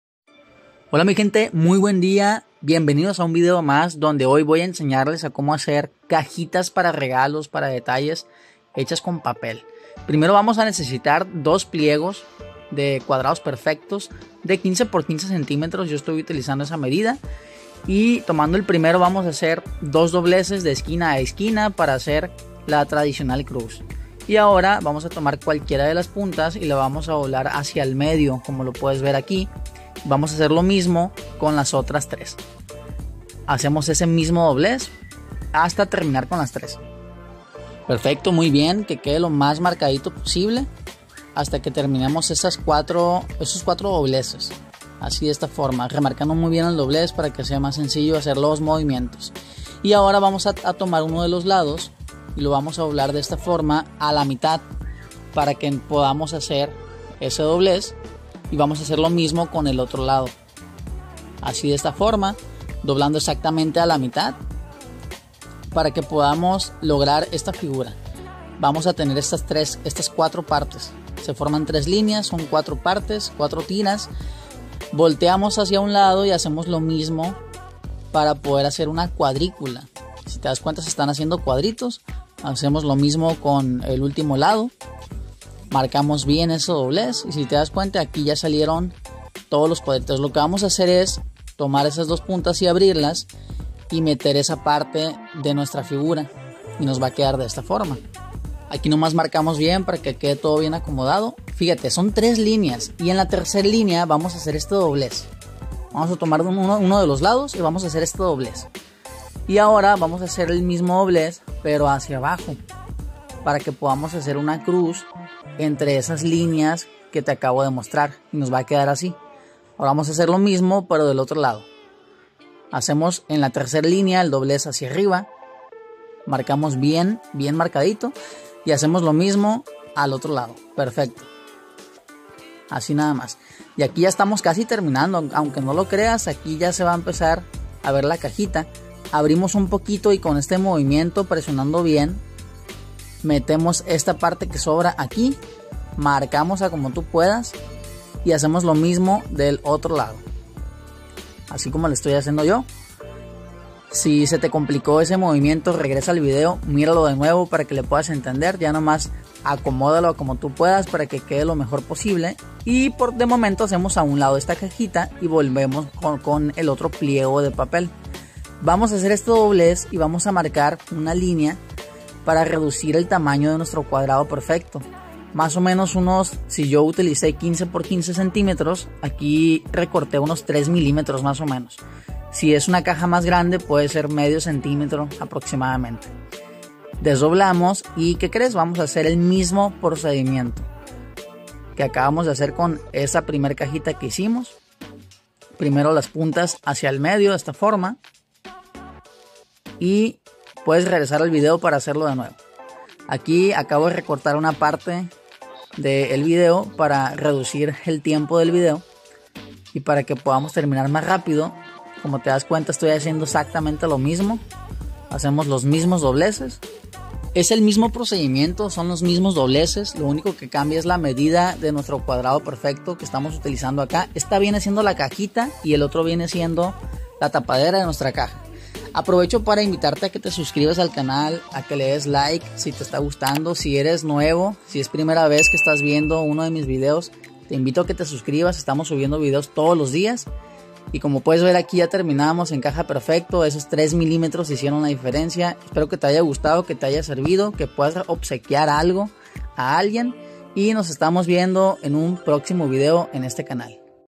🎁🎁 ¿CÓMO HACER CAJITAS PARA REGALO? EXPLICADO | ORIGAMI FACIL | MANUALIDADES LAS TIENES QUE HACER❗
Aquí te muestro como hacer cajas de regalo de papel de la manera más fácil y rápida posible.